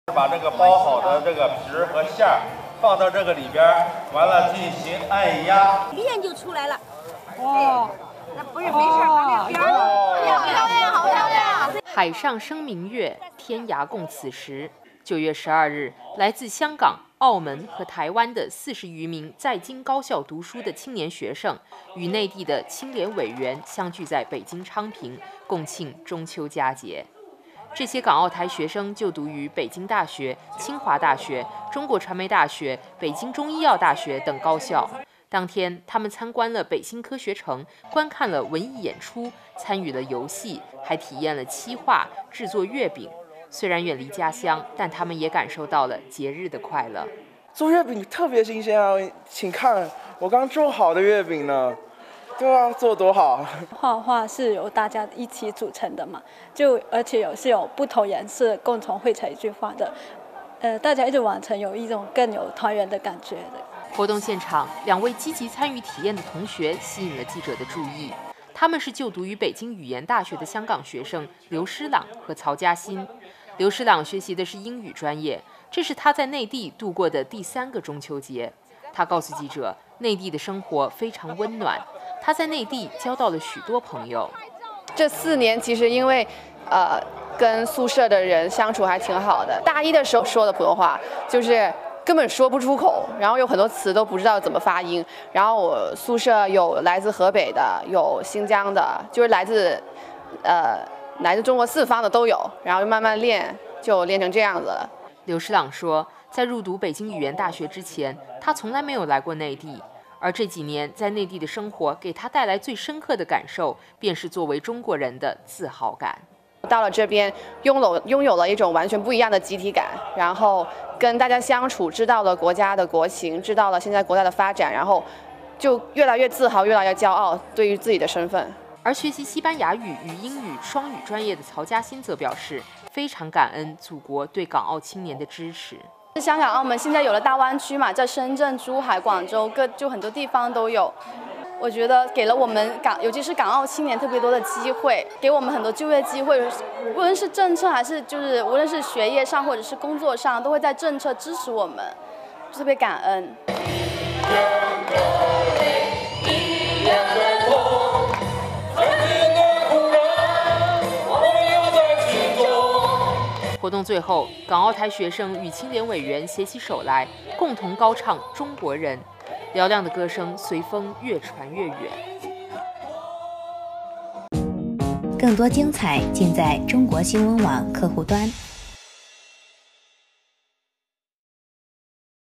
香港学生：在内地学习使我作为中国人越来越骄傲
“海上生明月，天涯共此时”。9月12日，来自香港、澳门和台湾的40余名在京高校读书的青年学生，与内地的青联委员相聚在北京昌平，共庆中秋佳节。有香港学生表示，在内地学习，拥有了一种完全不一样的集体感，跟大家相处，知道了国家的国情与发展，对作为中国人感觉越来越自豪，越来越骄傲。